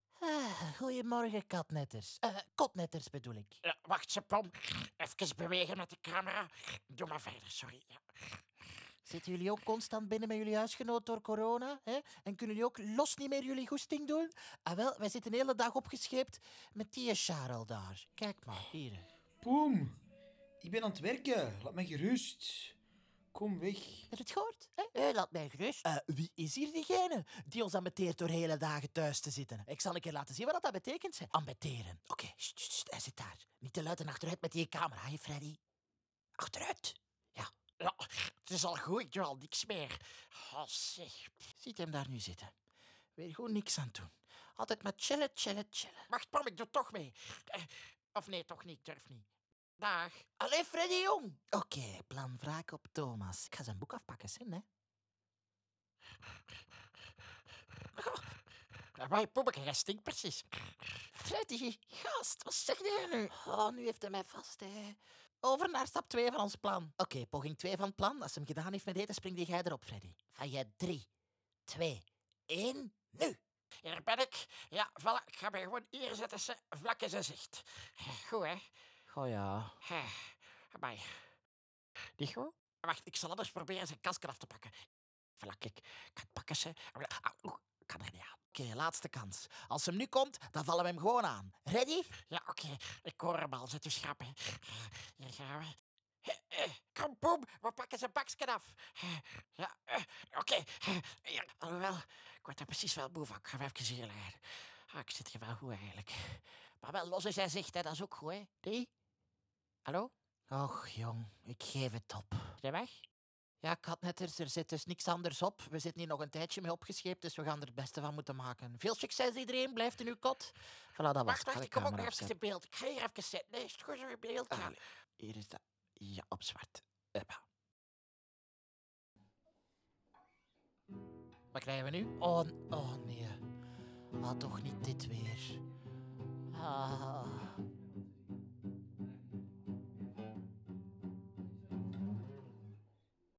kotnet: Freddie en Poemmeke nemen wraak op baasje Thomas
Freddie en Poemmeke nemen wraak op baasje Thomas